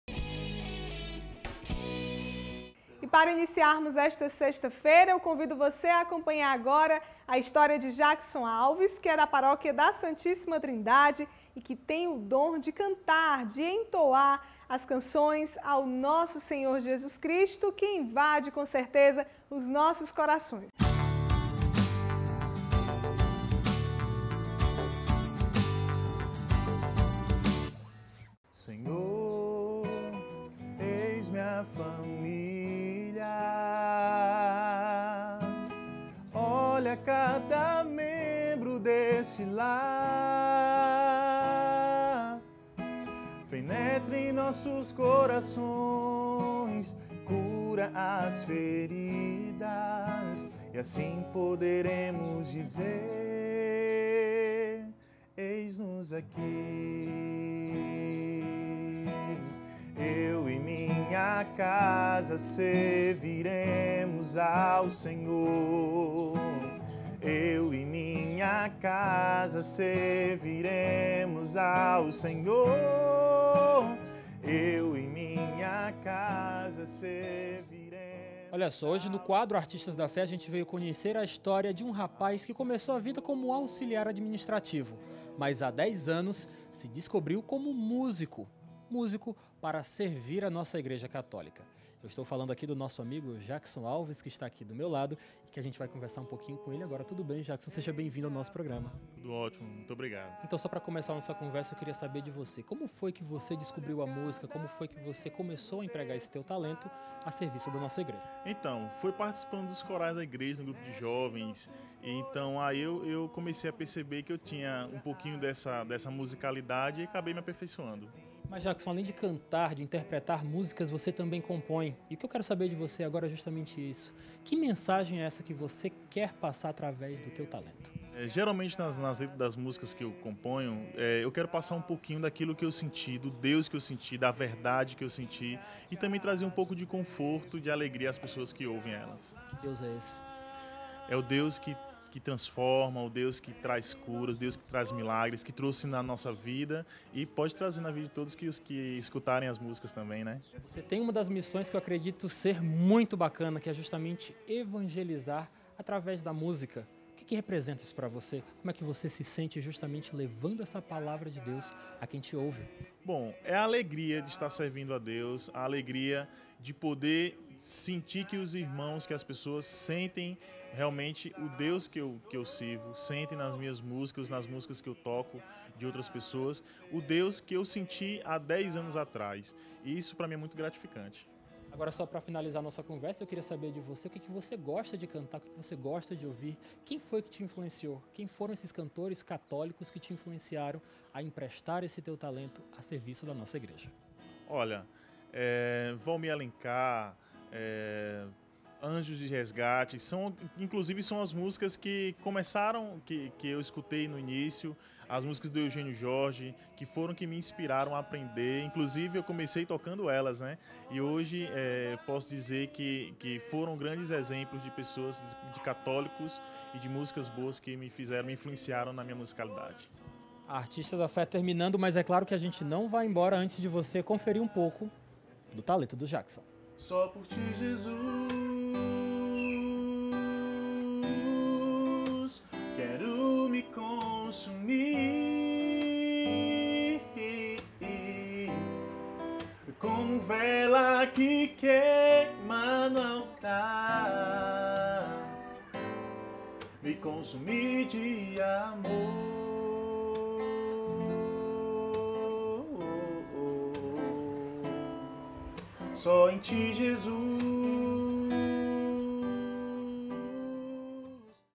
ETM 06 05 16 Artistas da Fé - Jackson Alves
Conheça mais uma história de um Artistas da Fé,  Jackson Alves, que há dez anos se descobriu como músico a serviço de Deus. Confira!